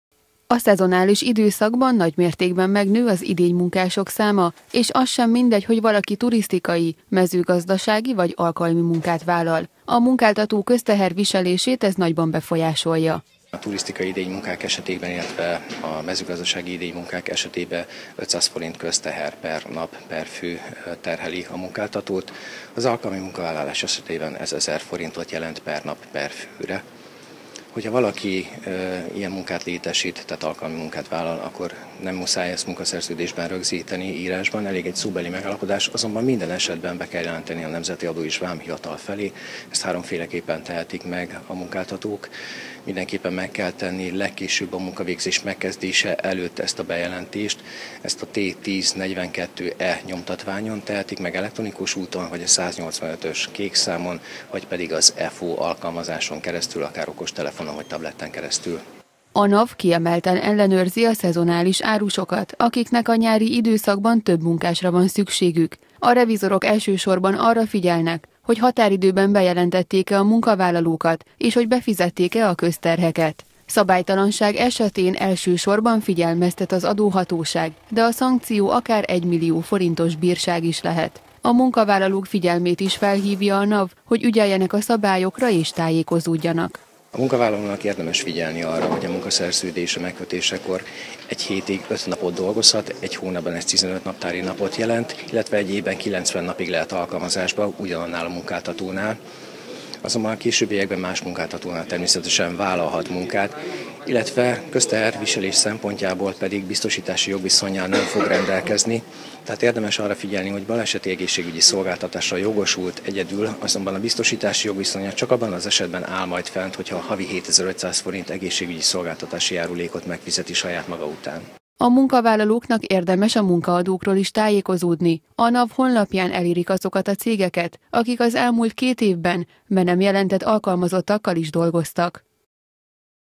Szabályosan bejelentik az alkalmi munkásokat - Kölcsey Televízió
Elérte a 269 ezret a bejelentett alkalmi munkások száma idén júniusban. Ez a szám öt évvel ezelőtt még csak 166 ezer volt. A legtöbb egyszerűsített foglalkoztatásban dolgozó a kereskedelemben helyezkedett el, de jellemző még ez a foglalkoztatási típus az építőiparban, a vendéglátásban és a mezőgazdaságban is, derül ki a NAV adataiból. Alkalmi munkák esetében egyébként a július számít csúcshónapnak.